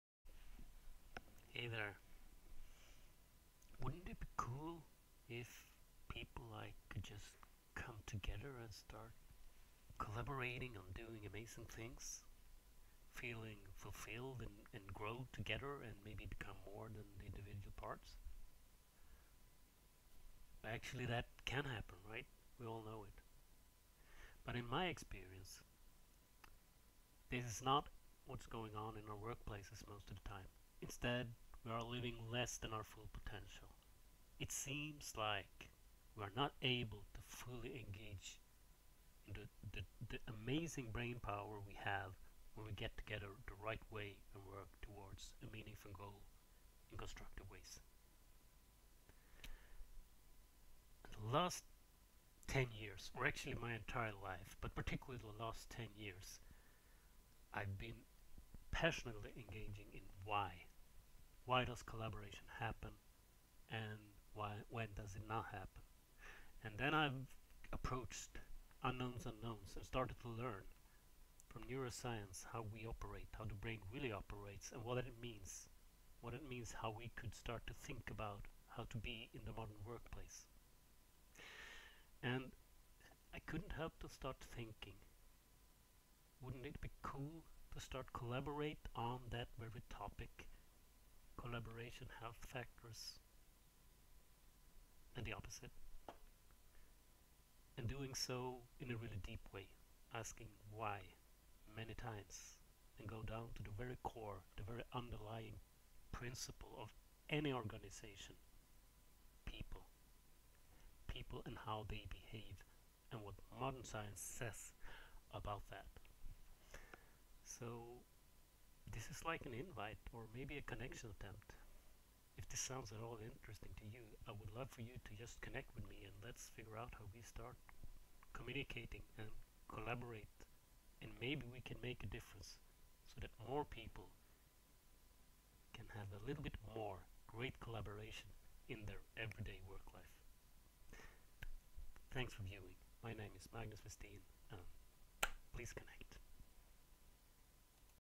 Collaboration - a first iteration
Over they years I've experienced a lot of pain and obstacles when it comes to collaboration at the workplace. The type of issues that really should not be there, considering how we people can collaborate so joyfully and easily with great results at other times. This is my first attempt to point at what I've learned so far.